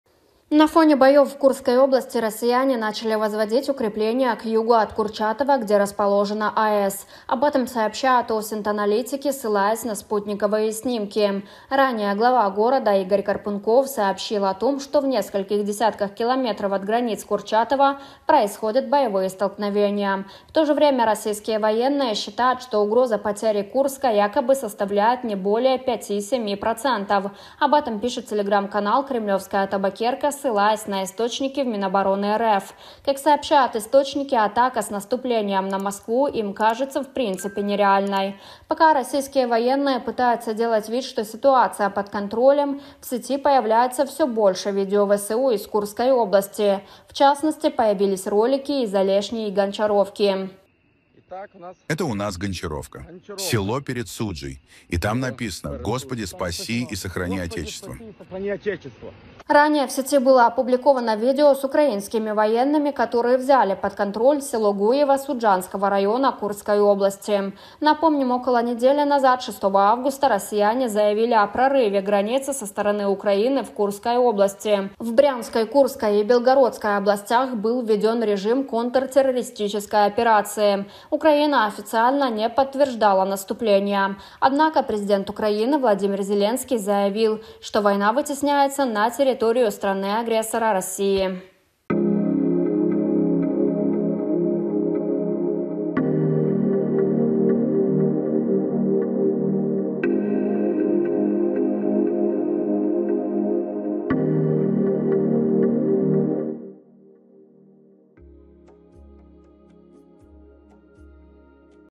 ⚡️ЧП на Курской АЭС: Курчатов готовят к обороне! Завязались бои, ВСУ все ближе
RU - Курскую АЭС начали активно готовить к обороне - бои приближаются к городу Курчатов, где находится станция.

UA - Курську АЕС почали активно готувати до оборони – бої наближаються до міста Курчатов, де знаходиться станція.